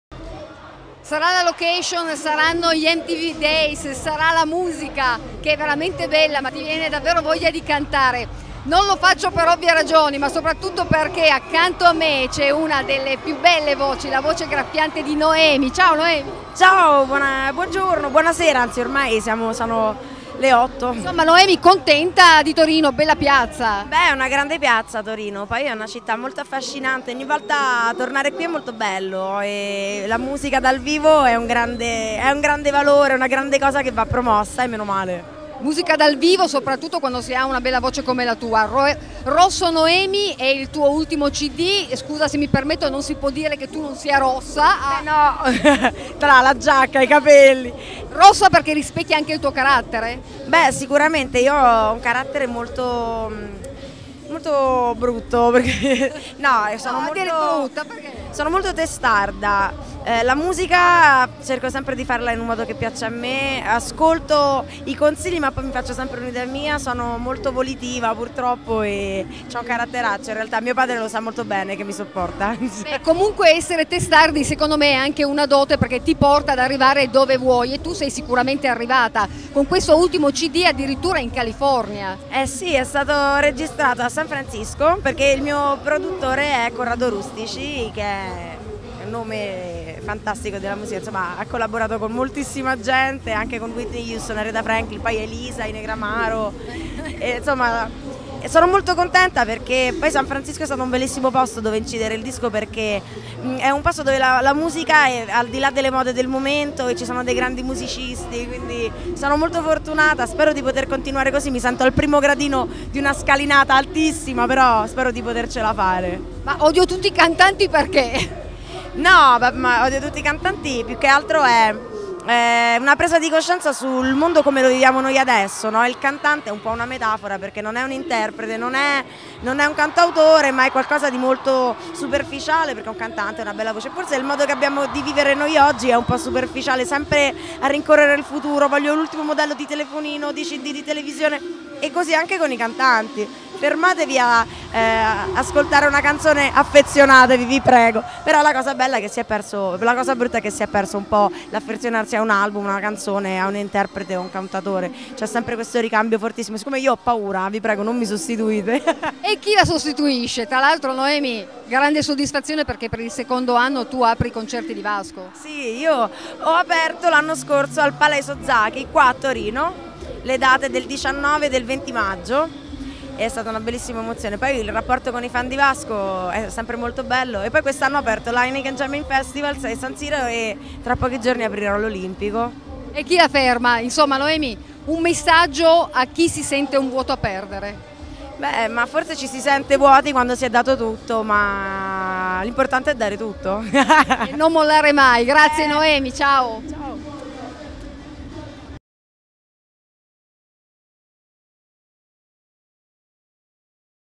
Noemi, la voce più graffiante agli Mtv Days
La voce graffiante di Noemi sul palco degli Mtv Days 2011: «Torino è una città affascinante, poter portare qui la musica live è un'occasione meravigliosa».

Intervista di Daniela Lanni (Agb)
Riprese di Andrea De Venuto
Servizio a cura di Dario Migliardi